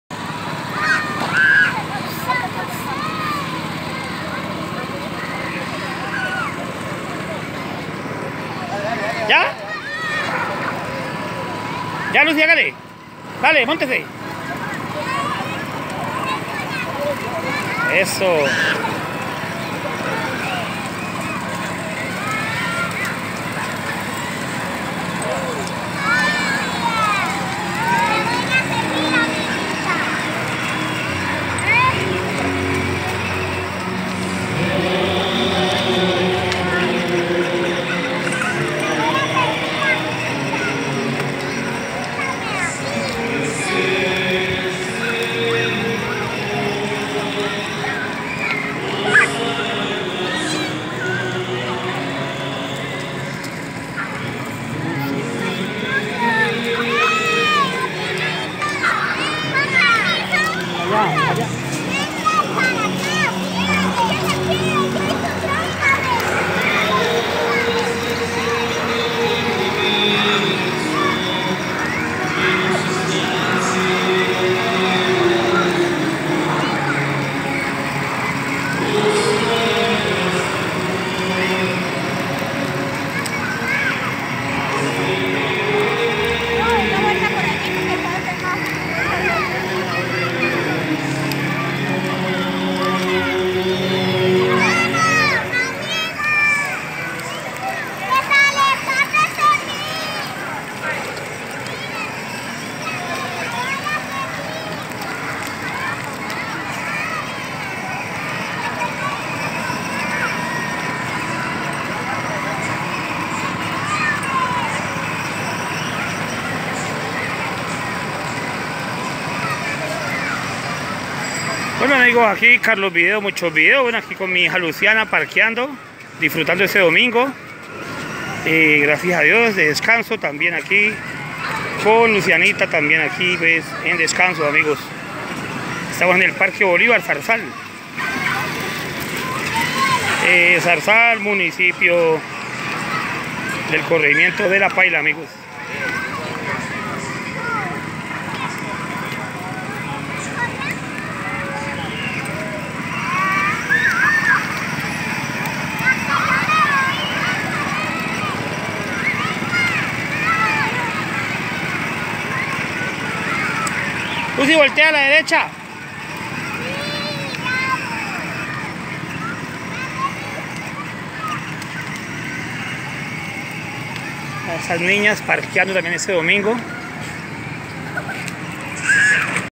zarzal Domingo en el parque bolívar
visitando zarzal parque bolívar con lucianita